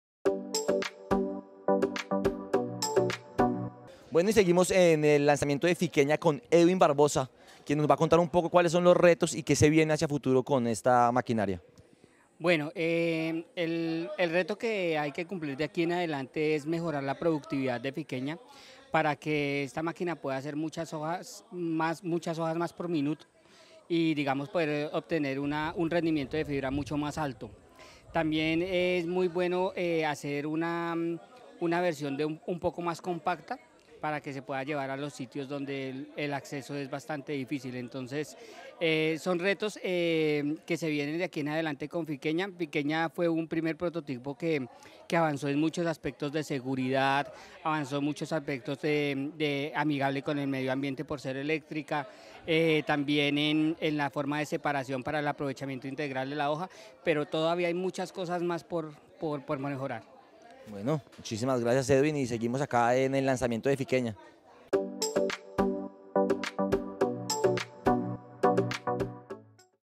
El investigador, Edwin Barbosa quien nos habló de las expectativas que se tienen con esta máquina
Conversamos en el lanzamiento de "Fiqueña" con el investigador máster, Edwin Barbosa quien nos habló de las expectativas que se tienen con esta máquina para la cadena del fique.